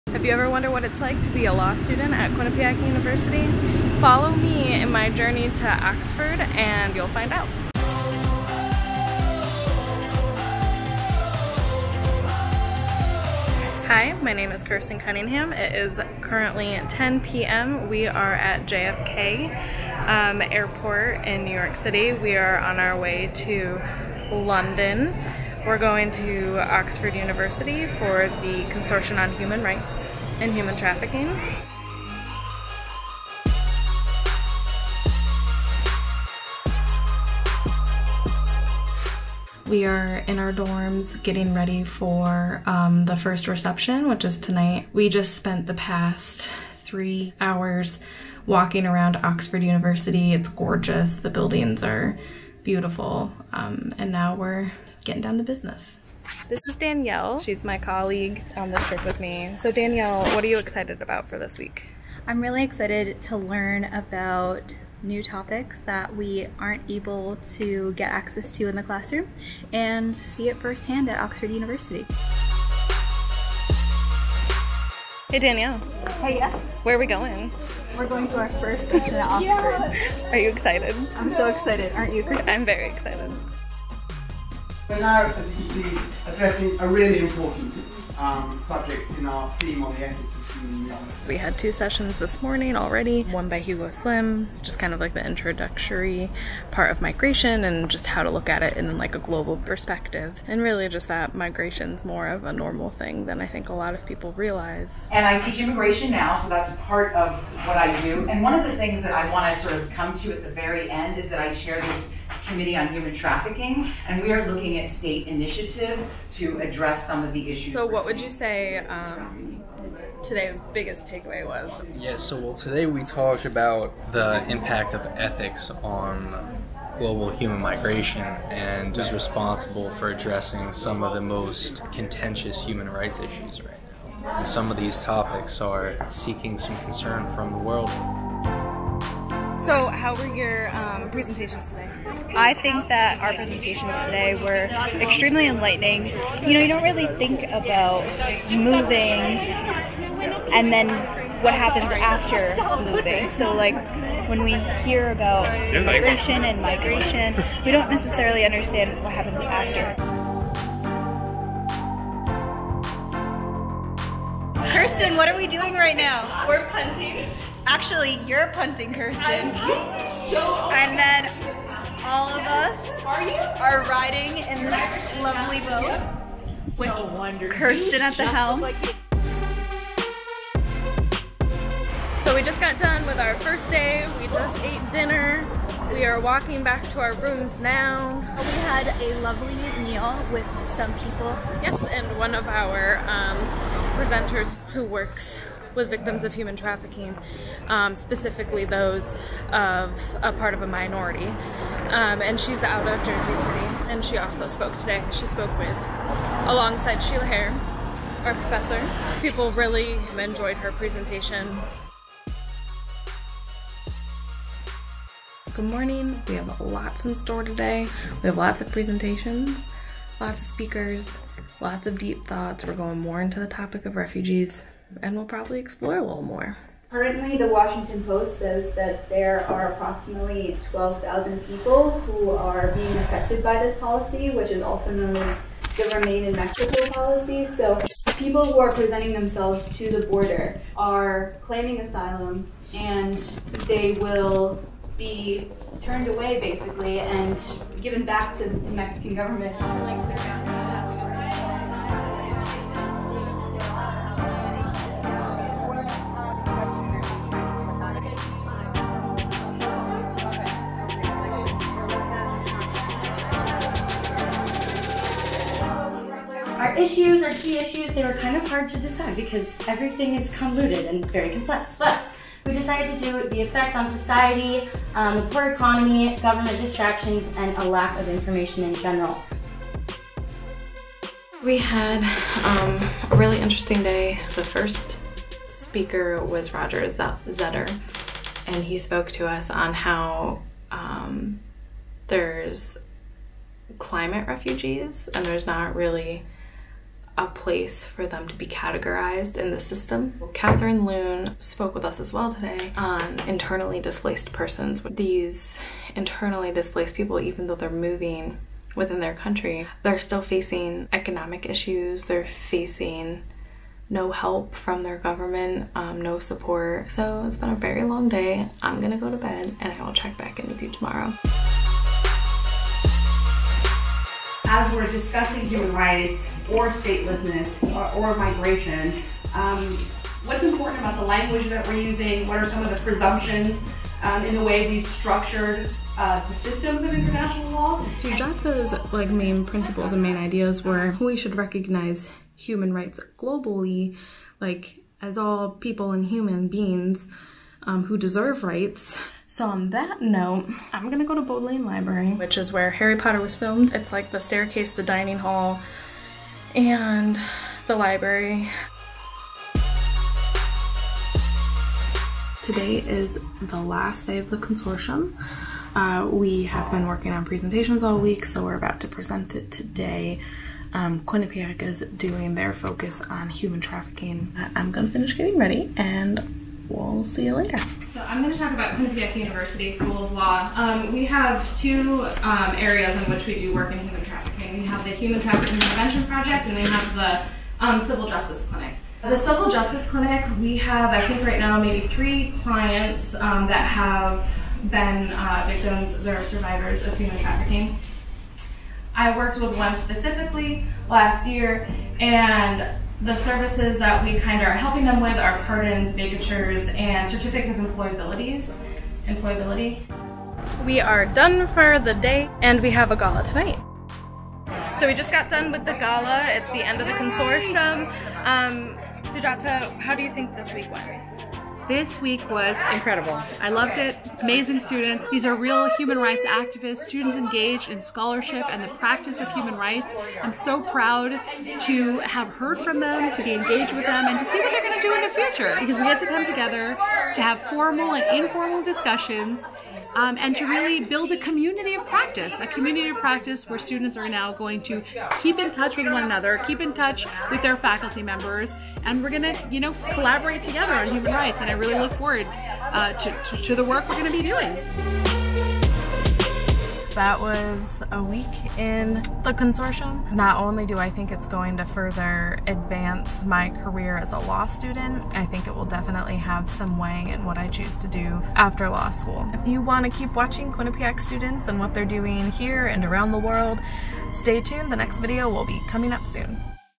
Day in the Life of a Law Student at the Oxford Consortium for Human Rights
Do you want to make an impact on human rights? At Quinnipiac, there are so many opportunities to guide you on your path to making a difference. Follow Kirsten Cunningham JD '20 as she takes you through her time at the prestigious Oxford Consortium for Human Rights.

The Oxford Consortium for Human Rights was a "workshop focused on the questions of human movement and global ethics as a framework for discussion of humanitarian action, conflict, human rights law, and peacemaking." Kristen, along with two other Quinnipiac students and their professors were part of a select group of participants for the workshop.

00:00 Meet Kirsten
00:39 First night at University of Oxford
01:12 First Sessions of the Consortium
01:57 Takeaways from the first day of the Consortium
02:40 Exploring Oxford with Friends
03:32 Beginning another day of learning and fun!
04:10 Working on a group project
04:40 Wrap up of the day
05:21 Starting another day of seminars, the importance of Human Rights
05:50 Exploring Bodleian Library
06:00 Last Day of the Consortium!
06:55 Gala and Thoughts about the Week
07:59 Looking for more?